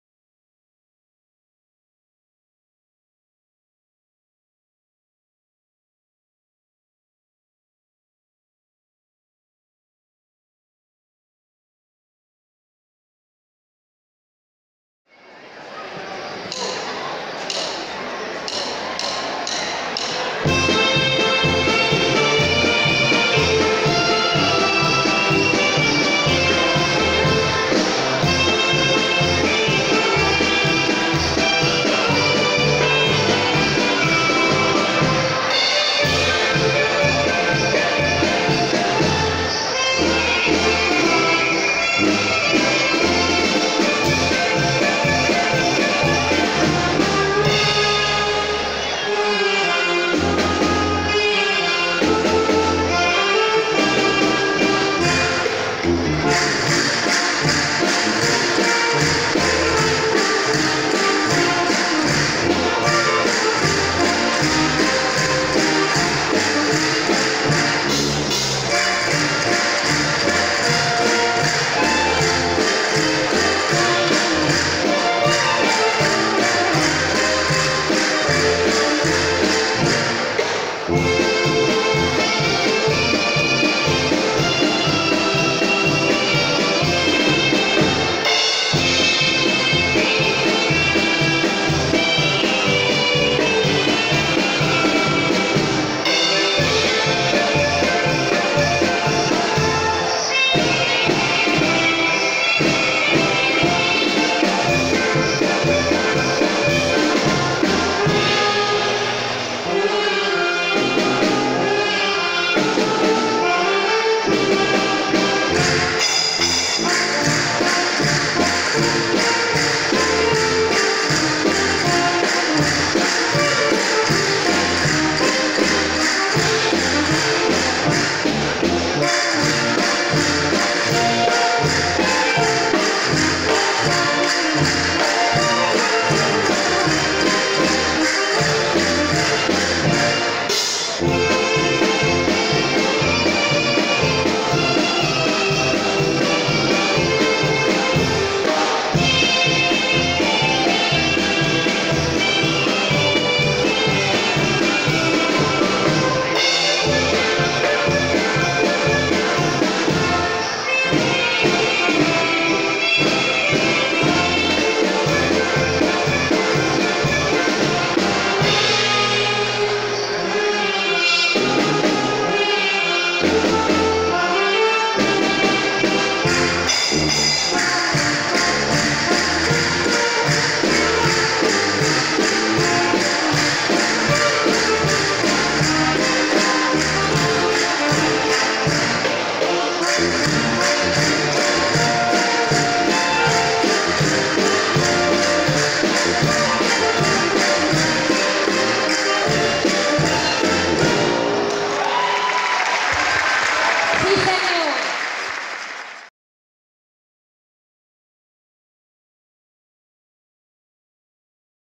Ball de Gitanes de Martorelles - Colla de Joves - " Contradanses " (02/04/2017)
Colla de Joves de Martorelles ballant les Contradanses a la ballada de roda de Martorelles  (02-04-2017)